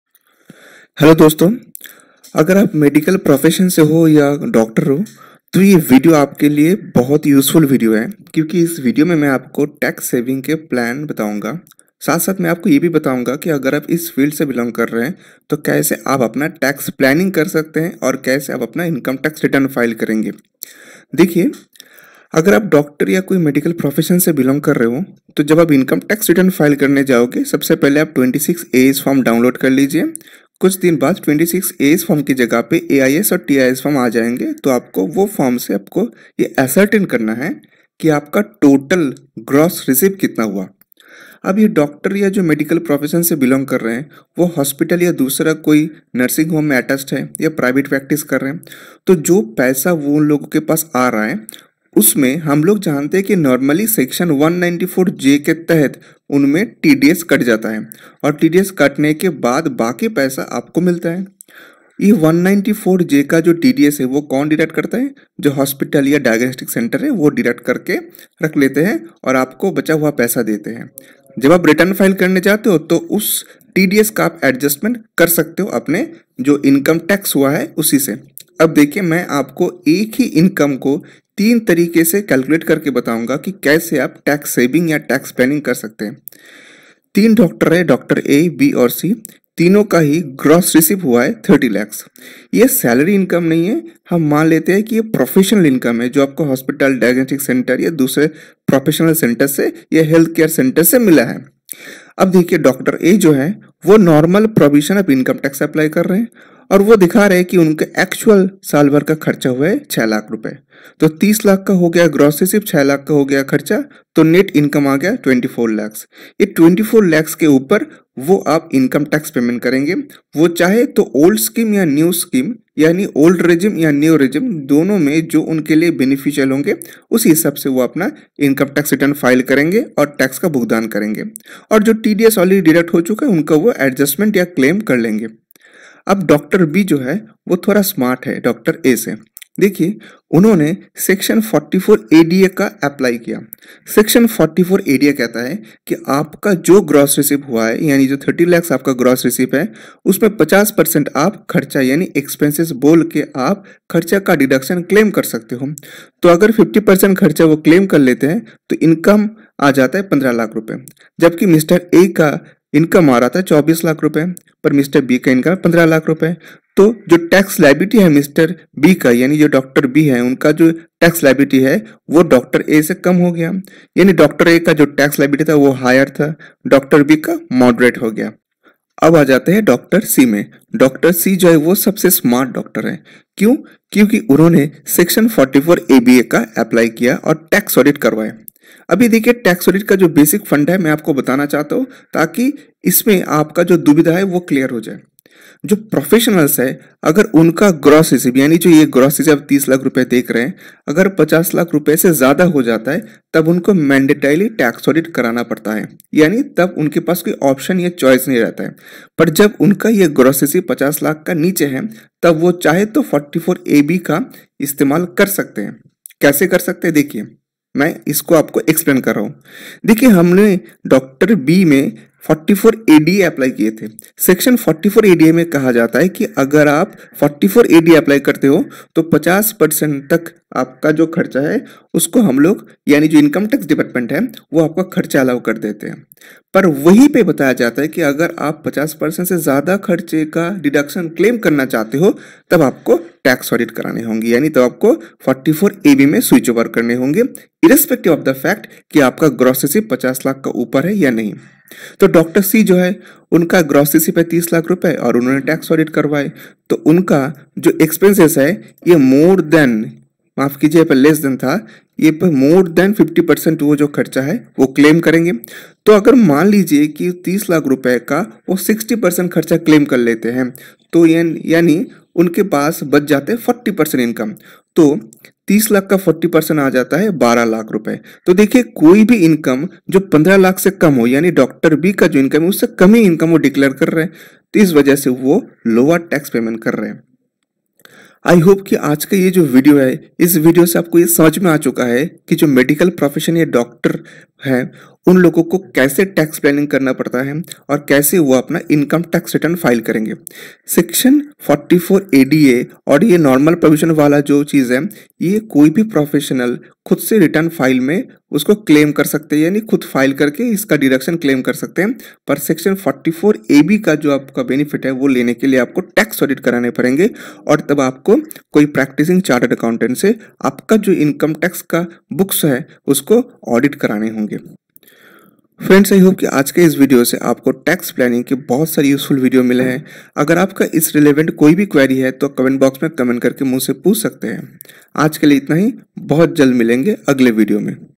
TAX PLANNING FOR DOCTOR / MEDICAL PROFESSION
This video states the simple way to save your tax liability if you belong to the profession of medical services.